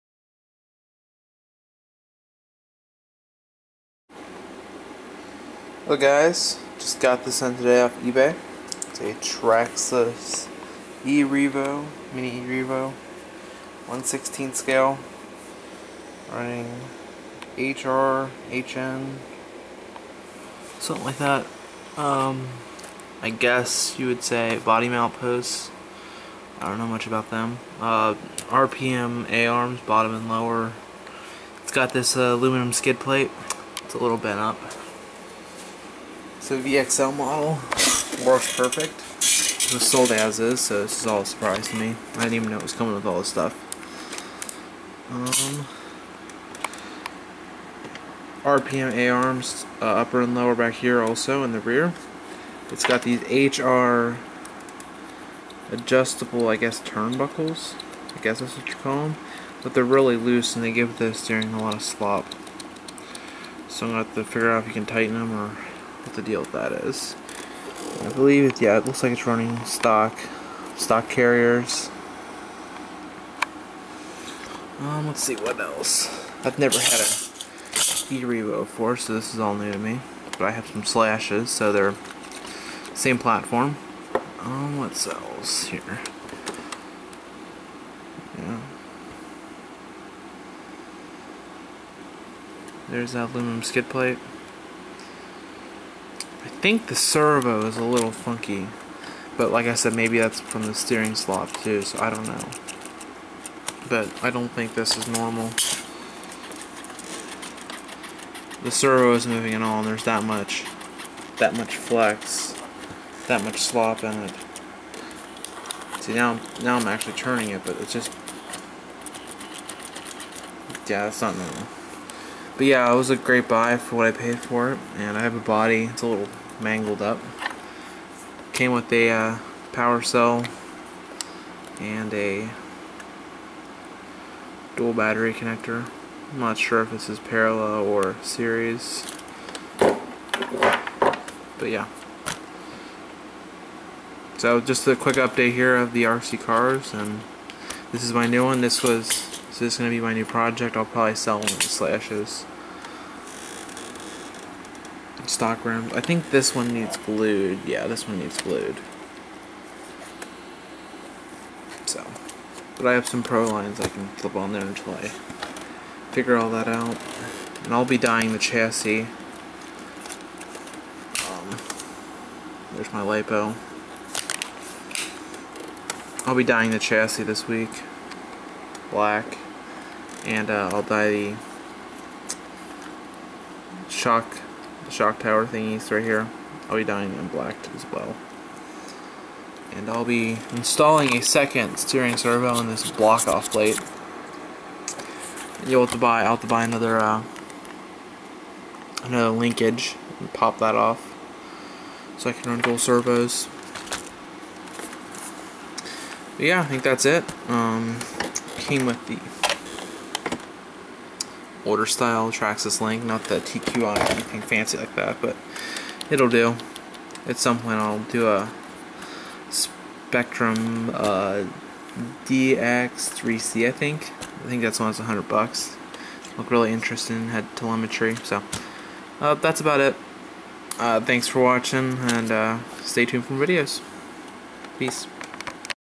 Update/Tour: I Got a mini E-Revo 1/16 VXL Off eBay with Aftermarket goodies!
Update: I Got a mini E-Revo 1/16 VXL Off eBay with Aftermarket goodies!